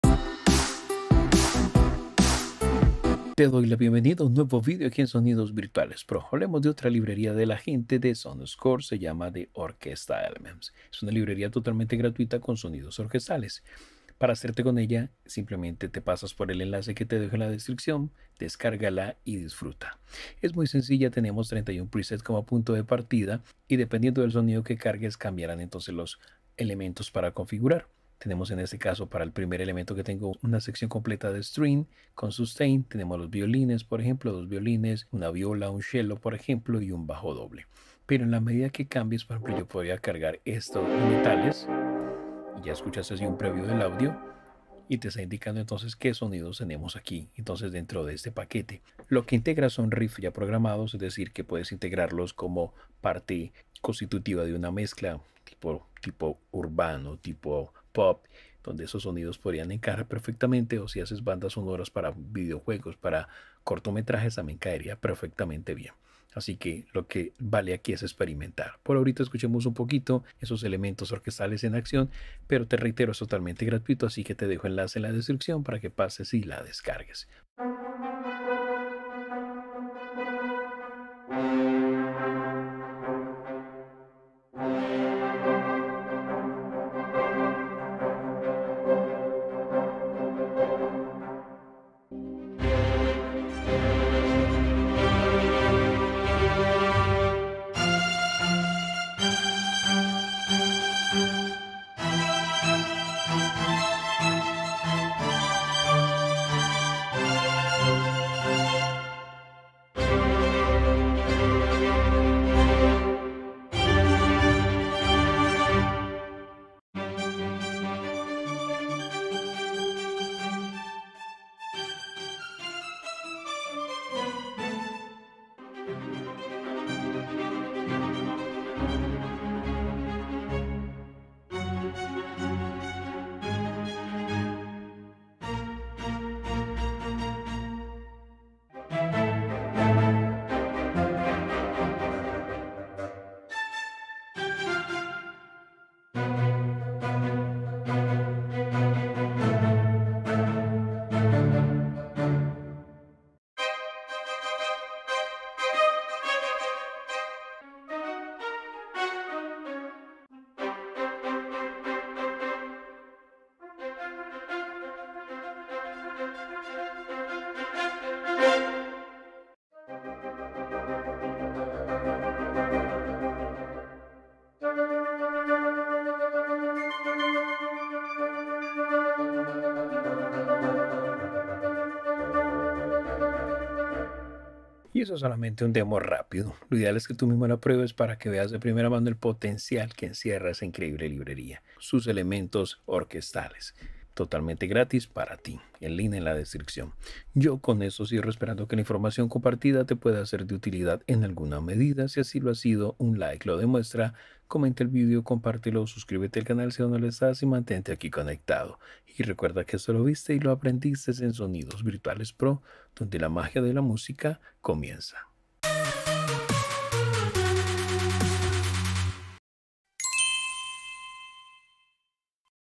🎻🎺🎷Sonuscore The Orquestra Elelemtents🎻🎺🎷 -Revisión en Español-¡GRATUITO!
Una librería para Kontakt cargada de sonidos orquestales y totalmente gratuita